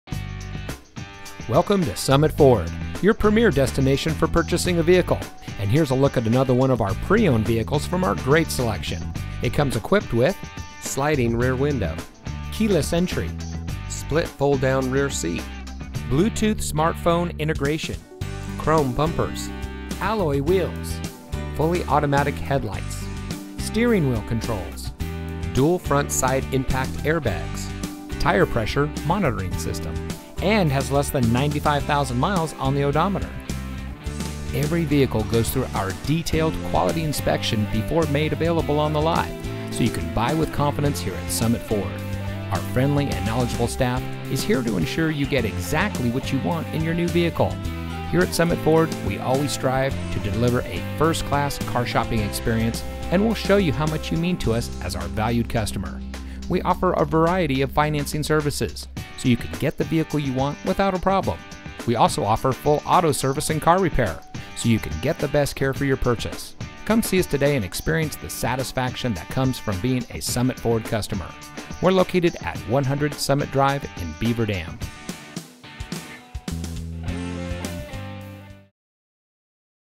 2017 Ford F-150 XLT SuperCrew Used. walk around for sale in Beaver Dam, Wisconsin
2017 Ford F-150 XLT SuperCrew Stock Number: 1084X Vin:1FTEW1EF4HFA22731